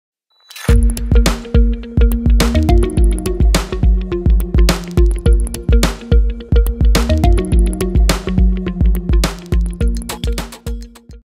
Pistol Squat to Box (1 Up 2 Down)
Category: Bodyweight
Target Muscles: Quads, Gluteus, Core